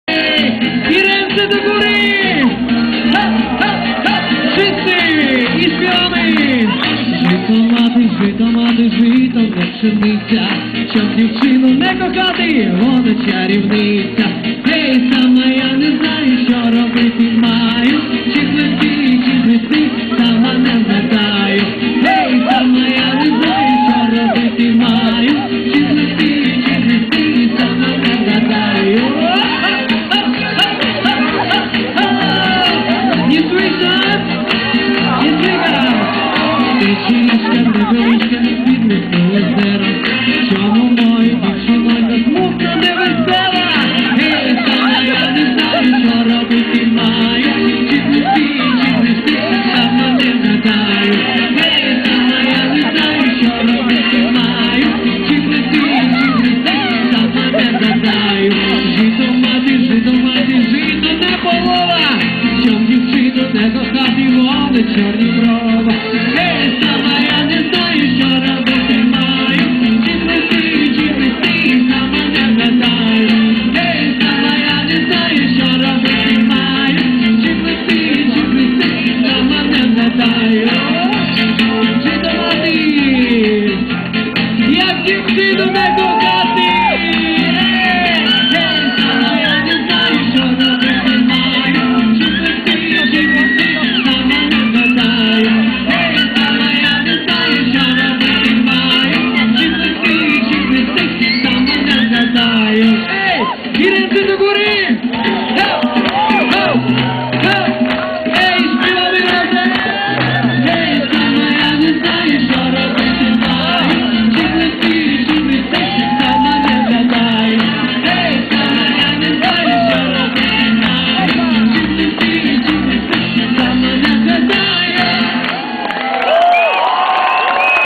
Festiwal Kapel i Śpiewaków Ludowych - Kazimierz Dolny 2004
Zabawa na zakończenie Festiwalu